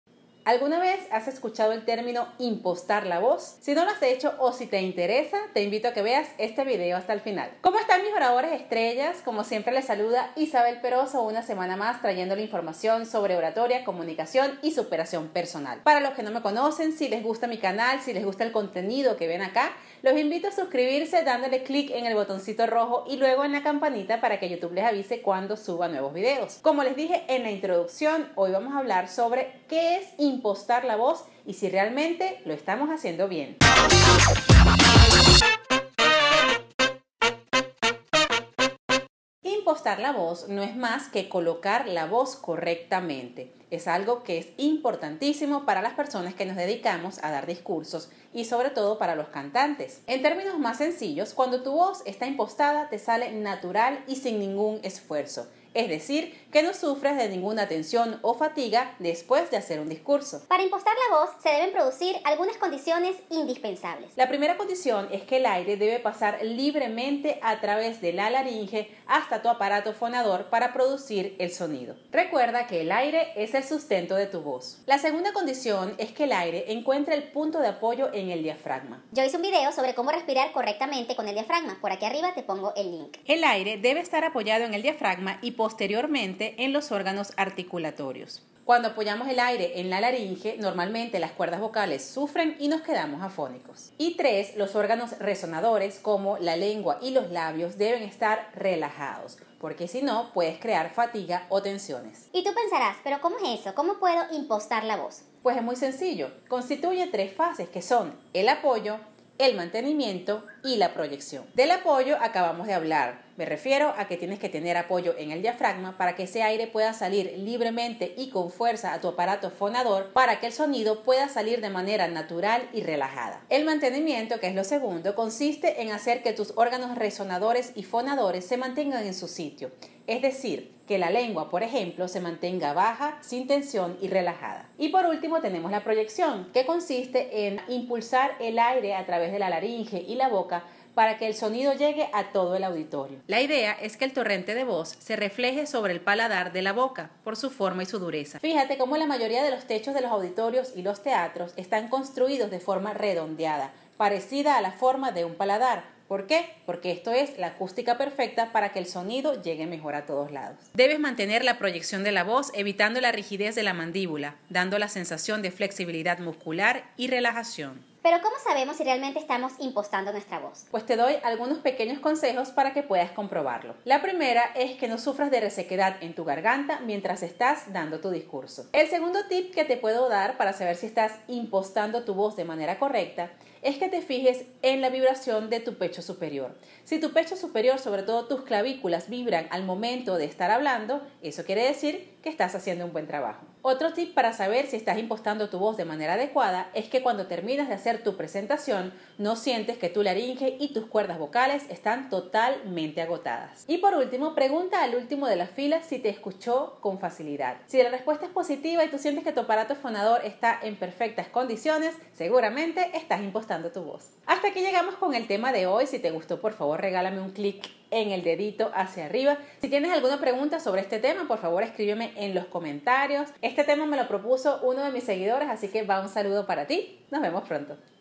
Cómo IMPOSTAR LA VOZ
Hola mis Oradores Estrellas! Esta semana vengo con un tema de Oratoria. Te enseñaré qué es Impostar la Voz y cómo lograrlo. Espero les sea útil!!!

Cámara y Diseño Gráfico: Argenis López @lopezargenis
Edición: Isabel Perozo